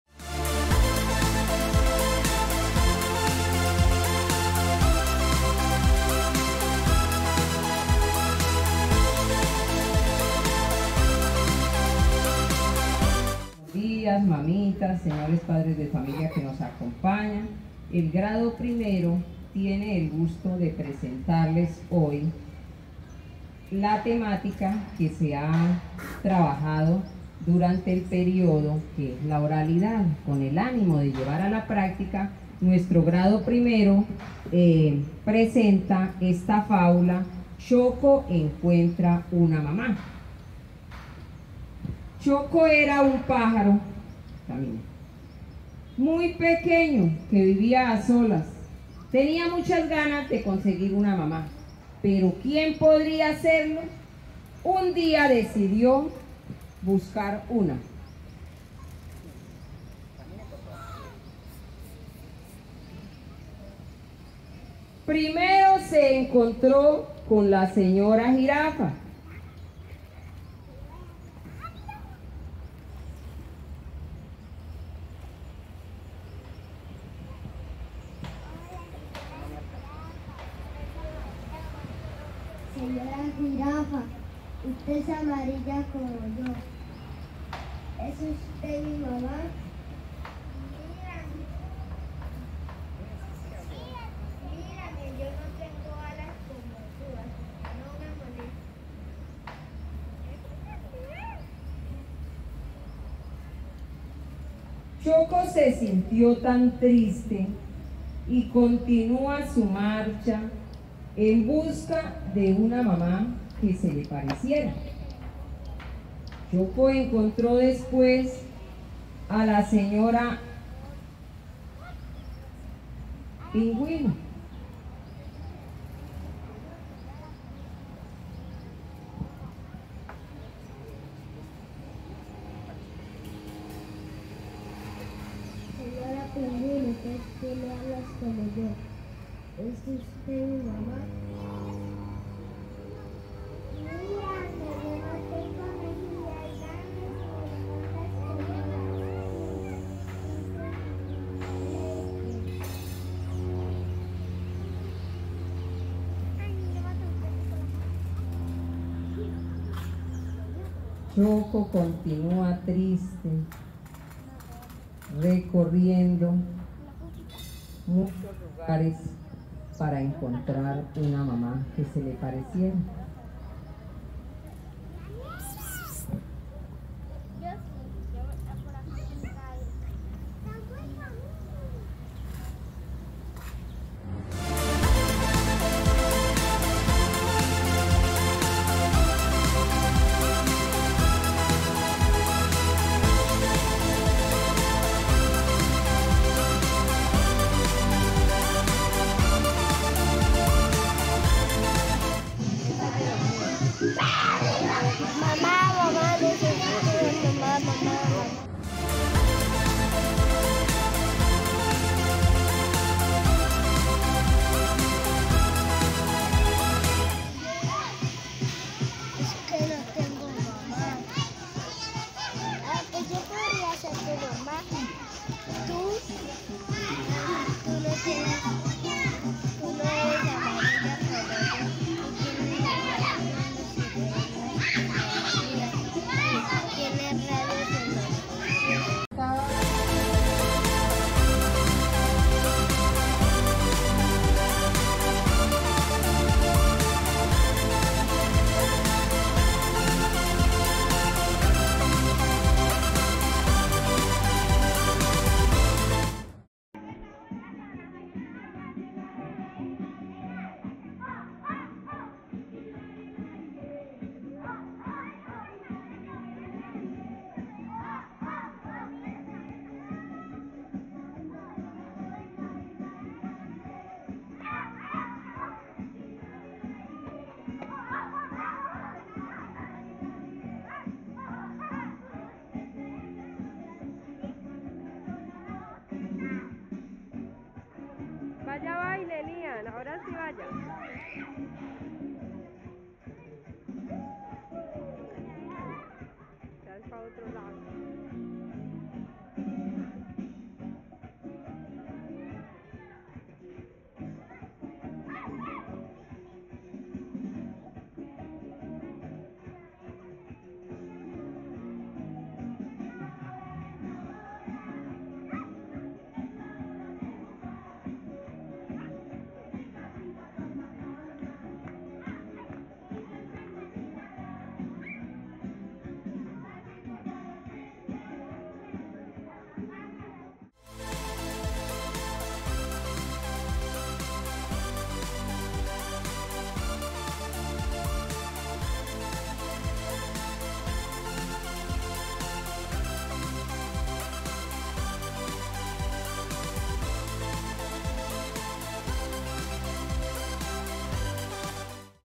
Cierre de proyecto ABPLA ORALIDAD grado primero sede D
Rescatando la tradición oral con la participación de sus familias en la lectura de cuentos y fábulas